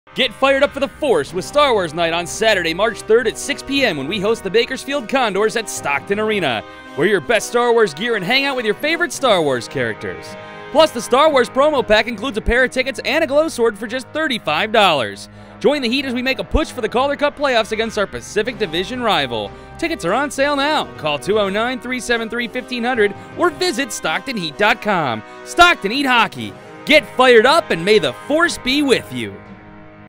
Star Wars Night | March 3rd
Star Wars Night with the Stockton Heat is coming to the Stockton Arena on Saturday, March 3rd as the Heat take on the Bakersfield Condors ! Come dressed as your favorite character from the iconic movie franchise and get "Fired Up by the Force!"